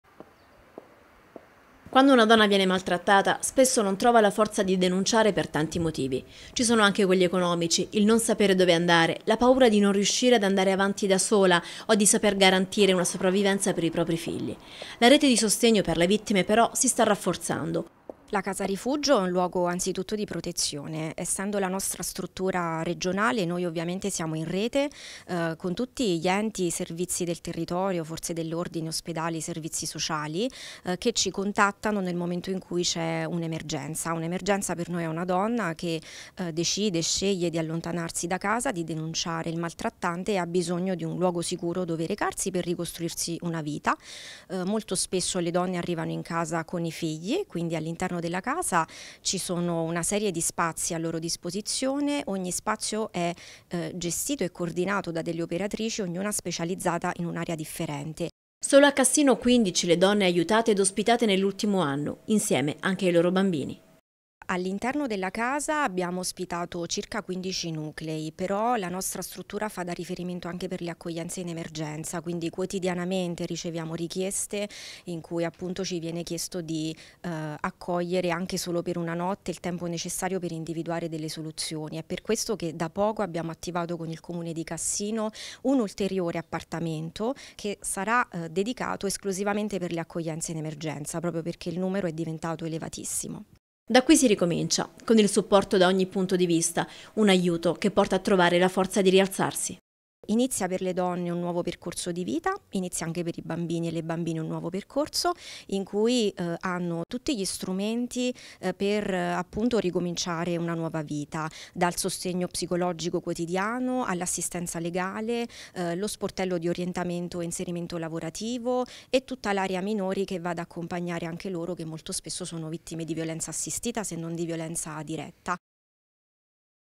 CASSINO: Casa rifugio risorsa donna del 17-10-2021
Un aiuto concreto alle donne vittime di violenza: un tetto sulla testa, per loro e per i loro bambini, un sostegno nella strada verso la rinascita. È quello che l'associazione Risorsa donna fornisce alle vittime, grazie anche alla casa rifugio inaugurata ormai da tempo a Cassino.